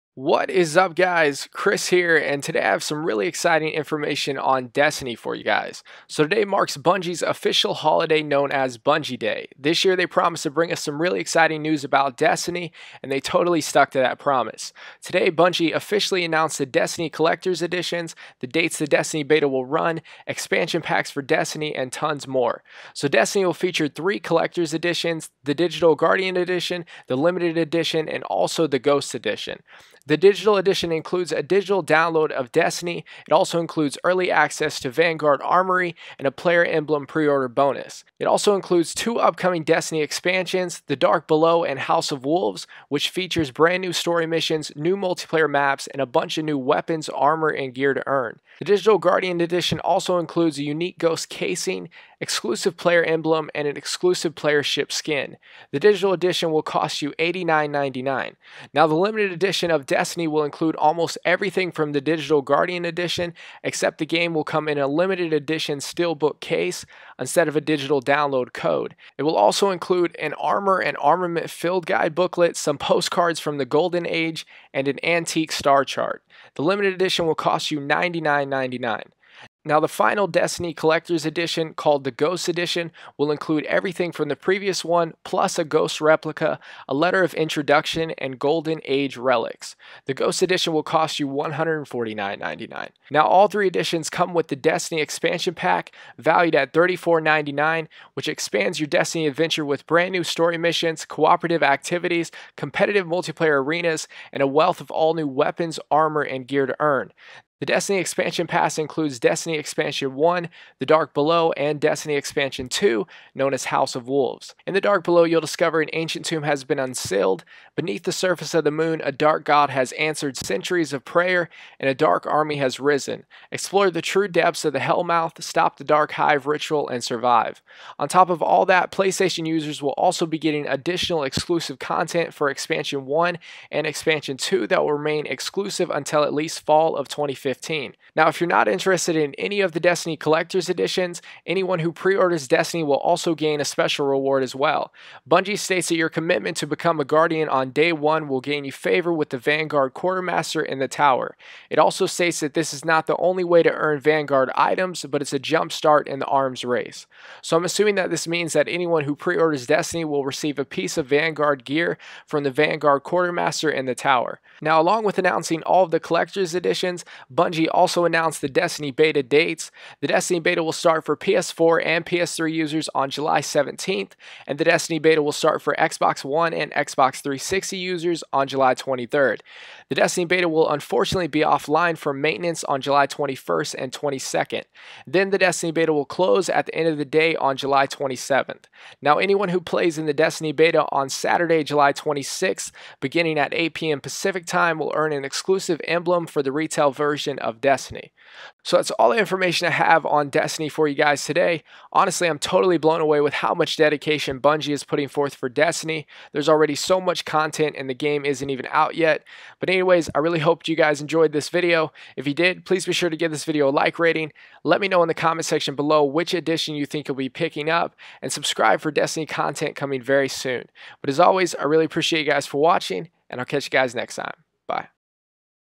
Destiny Collector's Edition, Expansion Pass, & Beta Dates!
In this video I cover the Destiny Collector's Editions: Digital Guardian Edition, Limited Edition, & Ghost Edition, including the Destiny Expansion Pass, which features, The Dark Below & House Of Wolves, as well as the Destiny Beta dates.